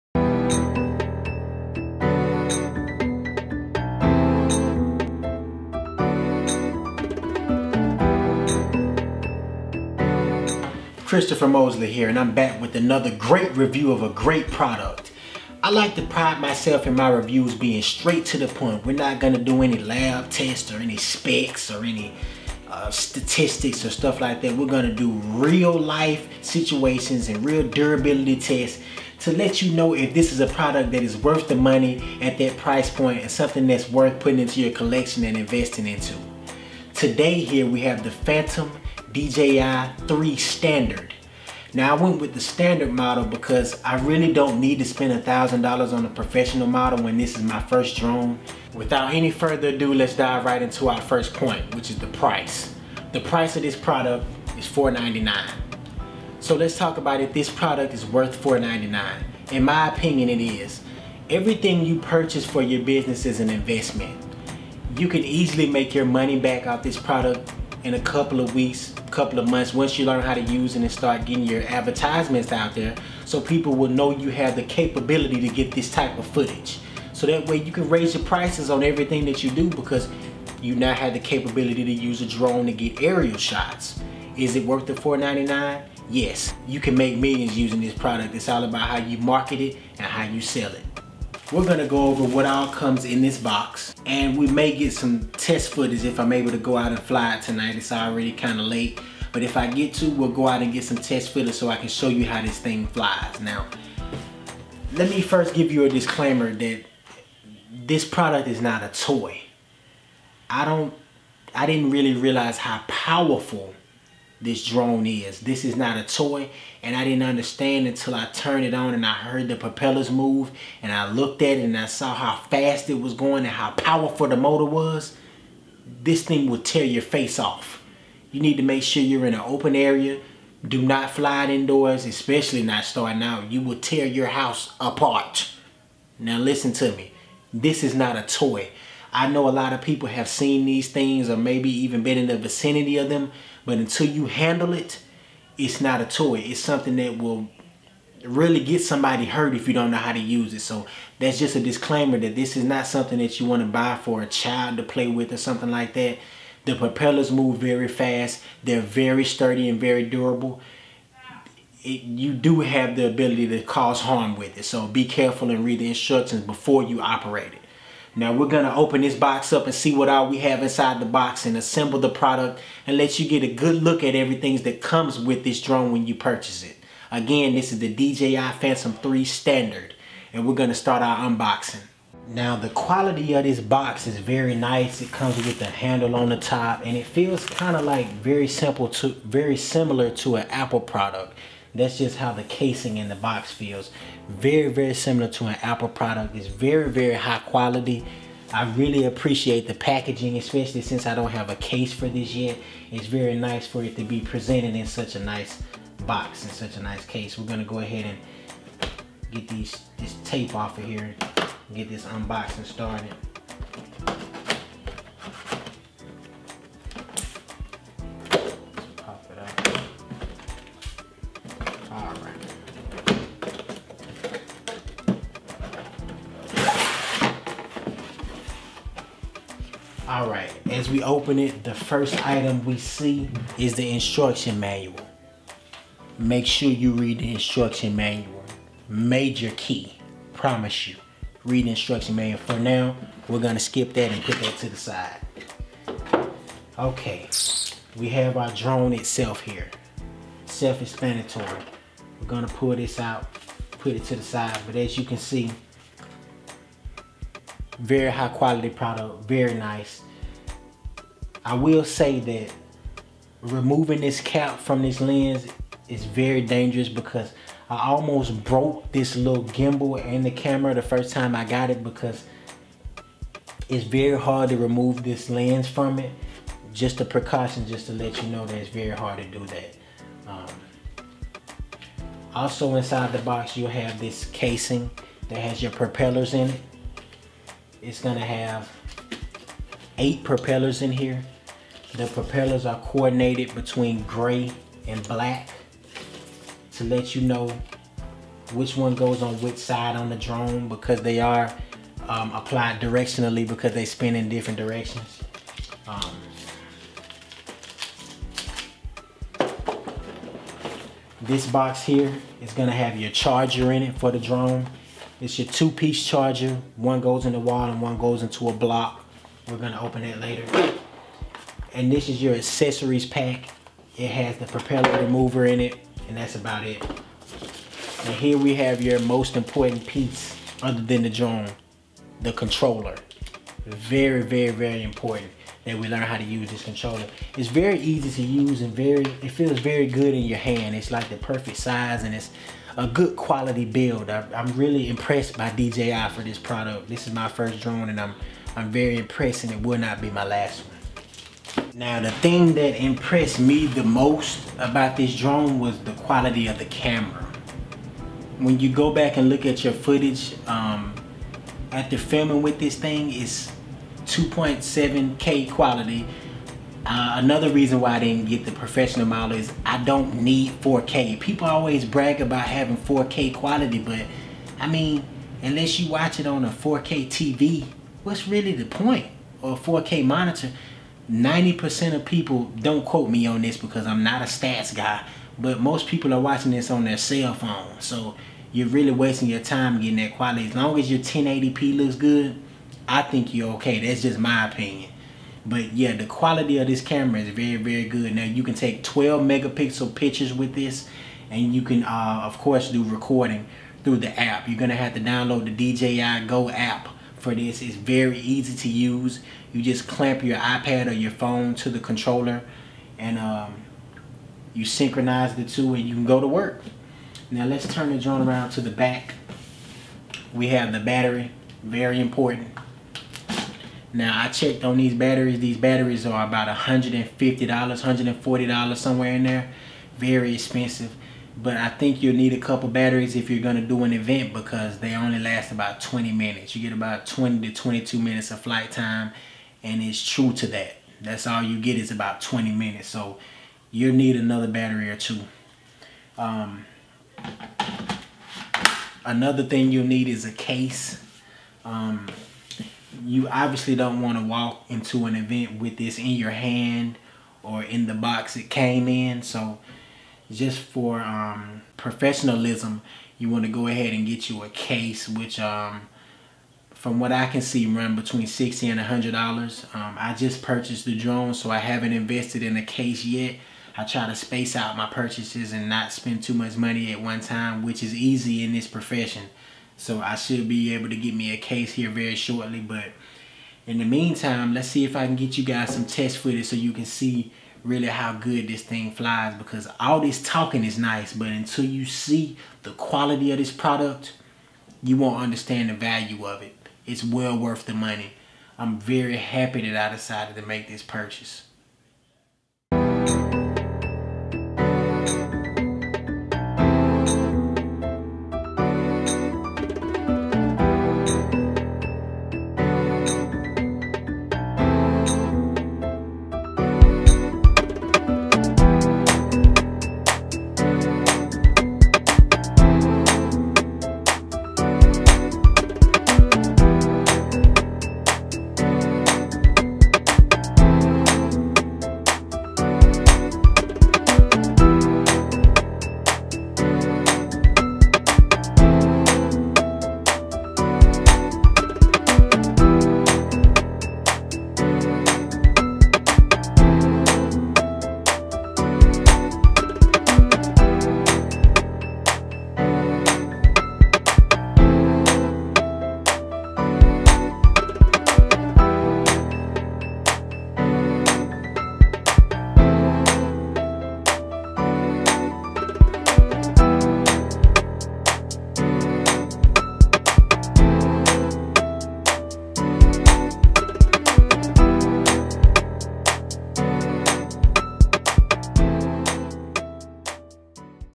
WHY YOU NEED A DRONE // DJI Phantom 3 Standard Unboxing/Review
DJI Phantom 3 Standard Unboxing/Review

Filmed By:

Real As It Gets Inc.
Facebook: Real As It Gets Inc.